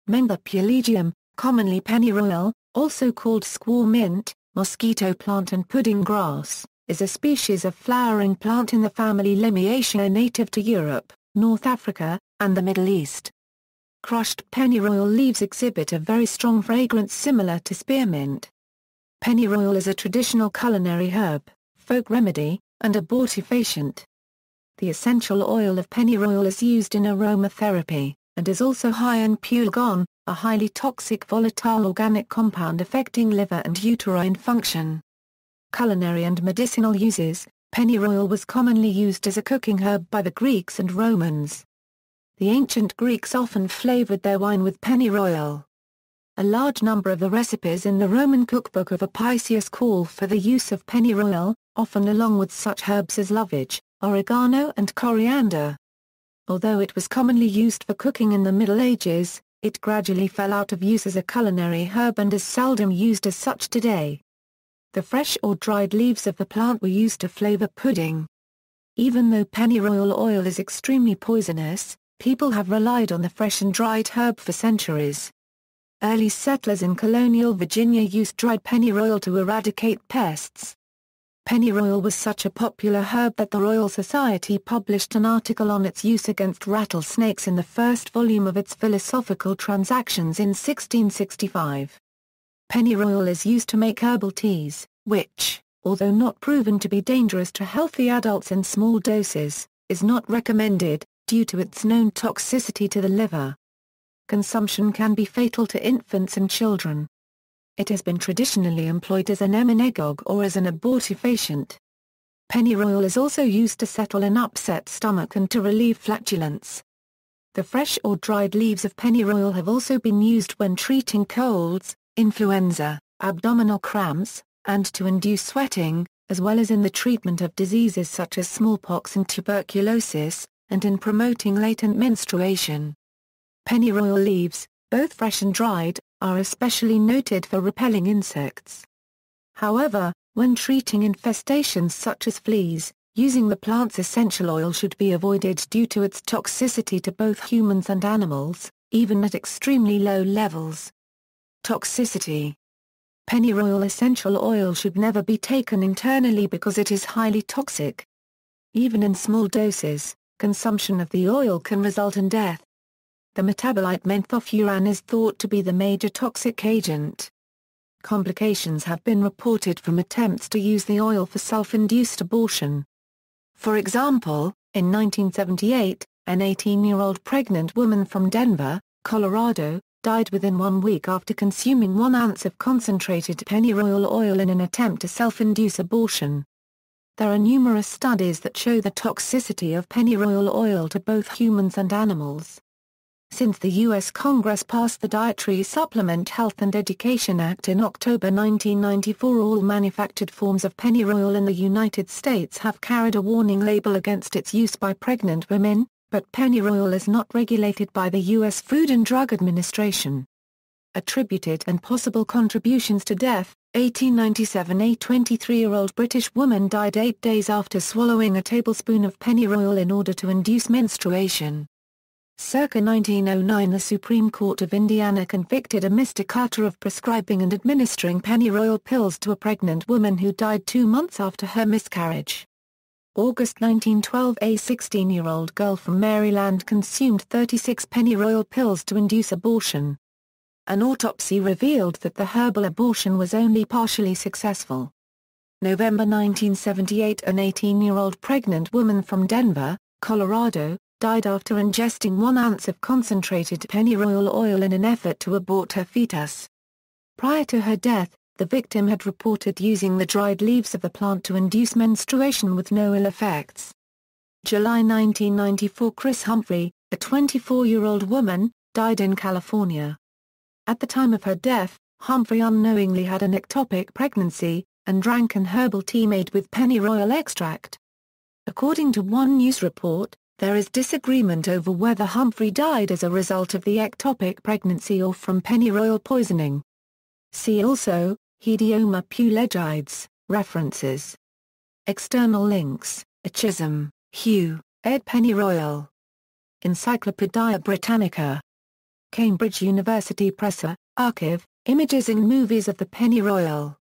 Mentha pulegium
Mentha pulegium, commonly (European) pennyroyal, also called squaw mint, mosquito plant and pudding grass, is a species of flowering plant in the family Lamiaceae native to Europe, North Africa, and the Middle East. Crushed pennyroyal leaves exhibit a very strong fragrance similar to spearmint. Pennyroyal is a traditional culinary herb, folk remedy, and abortifacient. The essential oil of pennyroyal is used in aromatherapy, and is also high in pulegone, a highly toxic volatile organic compound affecting liver and uterine function.